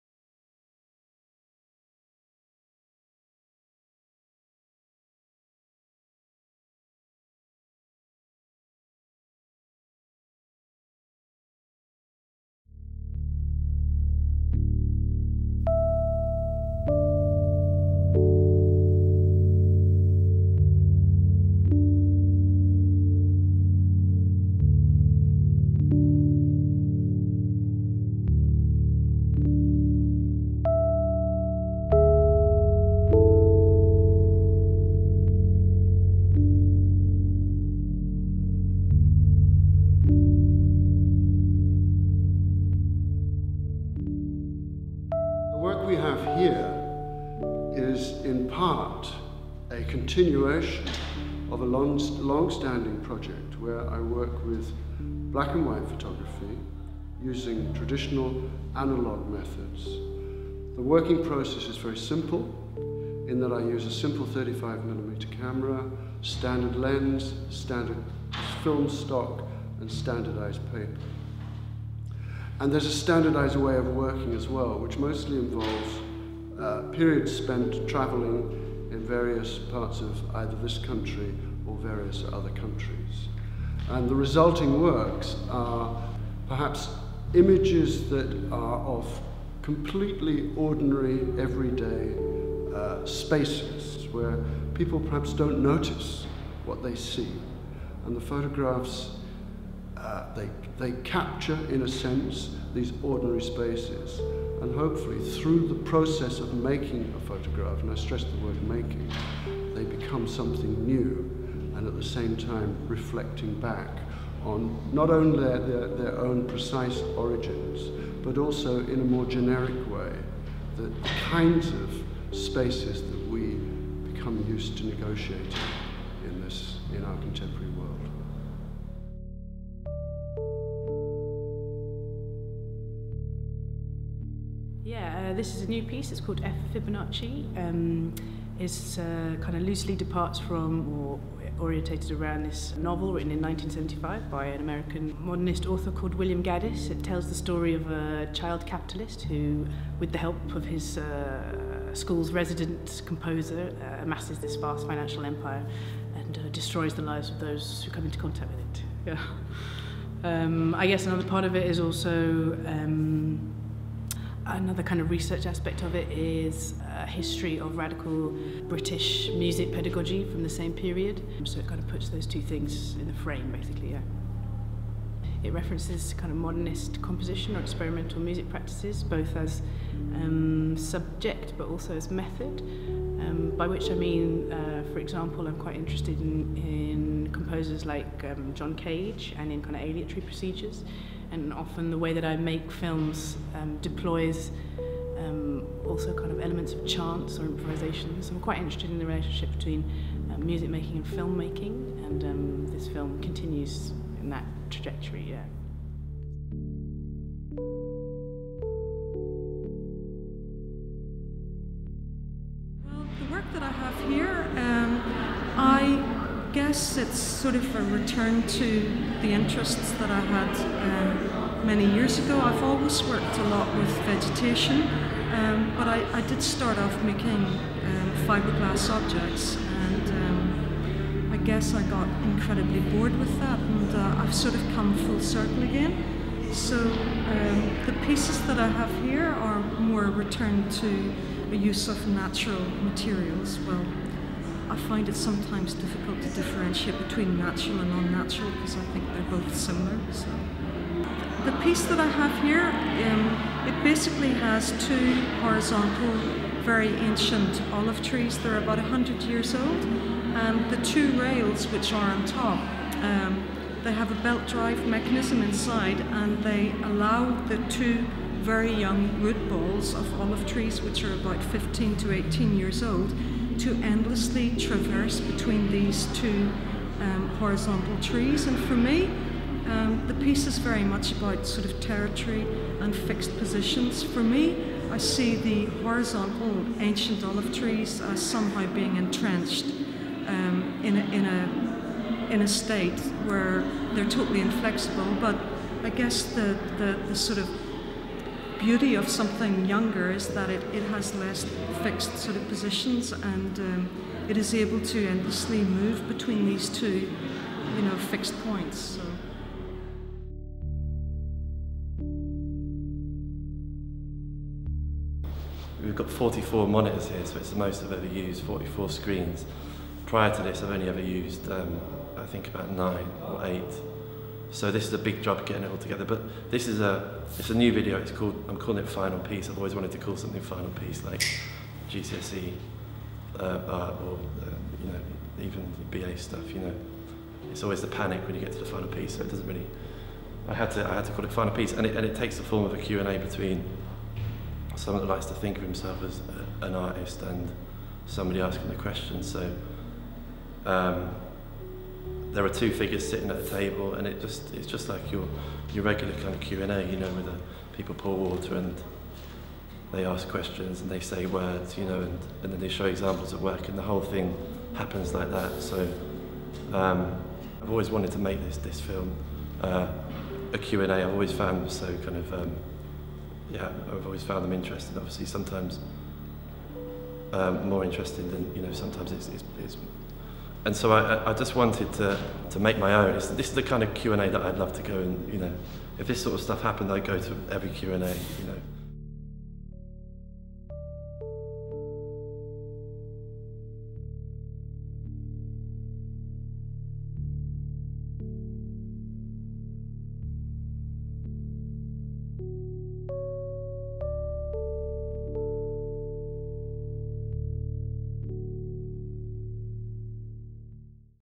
New City Art Prize / Exhibition
5 December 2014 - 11 January 2015

Beatrice Gibson, Siobhán Hapaska, Jimmy Merris and Roger Palmer

An exhibition of photography, sculpture, film and video by four leading artists in this inaugural £10,000 MK Community Foundation prize.

Film by Miles Umney